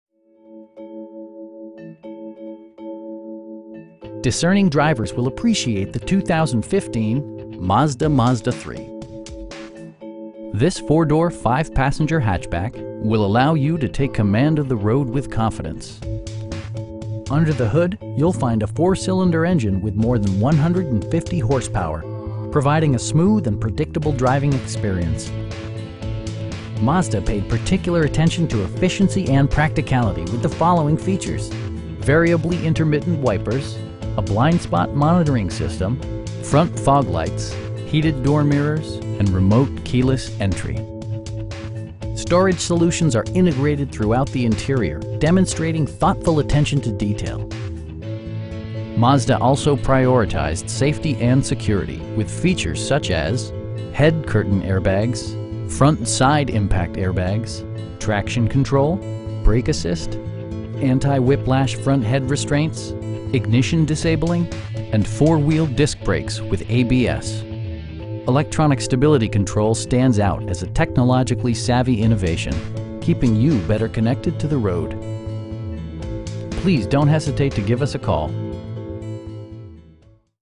2015 Mazda Mazda3 i in Santa Rosa, CA 95407
Hansel Ford

Discerning drivers will appreciate the 2015 Mazda Mazda3. This 4 door, 5 passenger hatchback will allow you to take command of the road with confidence! Under the hood you'll find a 4 cylinder engine with more than 150 horsepower, providing a smooth and predictable driving experience. Mazda paid particular attention to efficiency and practicality with the following features: a tachometer, variably intermittent wipers, a blind spot monitoring system, front fog lights, tilt and telescoping steering wheel, heated door mirrors, and remote keyless entry. Storage solutions are integrated throughout the interior, demonstrating thoughtful attention to detail. Mazda also prioritized safety and security with features such as: head curtain airbags, front SIDE impact airbags, traction control, brake assist, anti-whiplash front head restraints, ignition disabling, and 4 wheel disc brakes with ABS. Electronic stability control stands out as a technologically savvy innovation, keeping you better connected to the road. Our aim is to provide our customers with the best prices and service at all times. Please don't hesitate to give us a call.